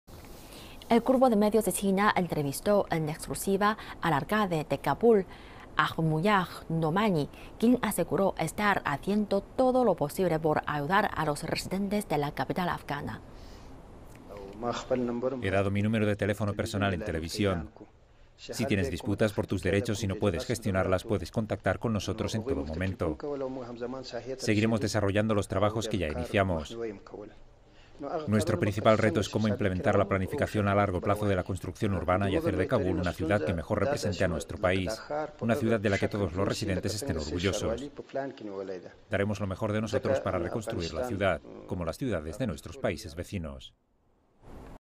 Entrevista en exclusiva con el alcalde de Kabul
El Grupo de Medios de China entrevistó, en exclusiva, al alcalde de Kabul, Hamdullah Nomani, quien aseguró estar haciendo todo lo posible por ayudar a los residentes de la capital afgana.